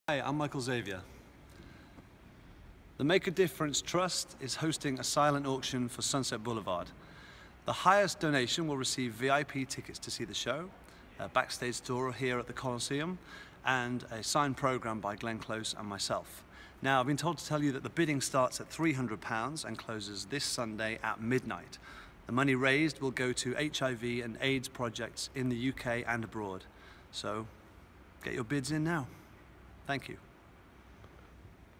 Sunset Boulevard Silent Auction for MAD Trust
The Make A Difference Trust hosting a Silent Auction for Sunset Boulevard! The highest donation will receive a pair of VIP tickets to the show, not only will you get terrific seats to see this amazing show starring the Tony-award winning actress Glenn Close and Olivier Award nominated Michael Xavier, you will also be given an exclusive backstage tour of the Coliseum, home of the English National Opera company, and a signed Programme by Glenn Close & Michael Xavier.

Bidding starts at £300 and closes this Sunday at midnight. MAD Trust works to bring together the British Entertainment community and its audiences to raise funds for, and support, people living with HIV & AIDS both in the UK and Southern Africa.

Get your bids in now folks!